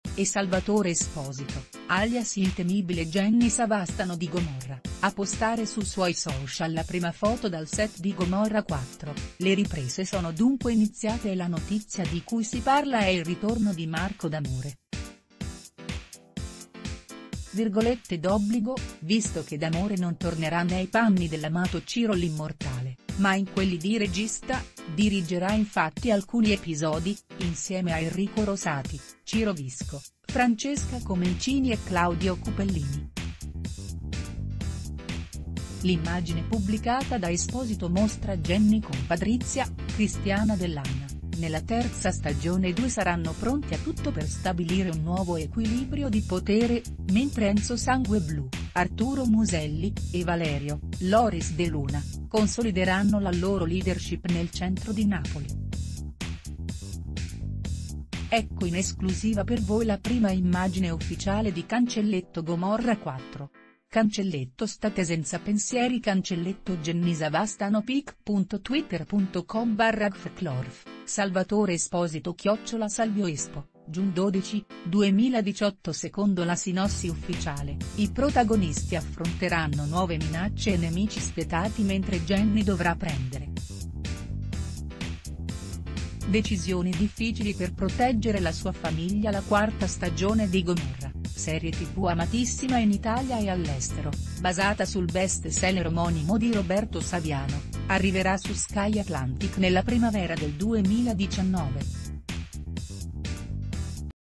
Gomorra 4, la prima immagine ufficiale di Genny Savastano
E’ Salvatore Esposito, alias il temibile Genny Savastano di Gomorra, a postare sui suoi social la prima foto dal set di Gomorra 4: le riprese sono dun...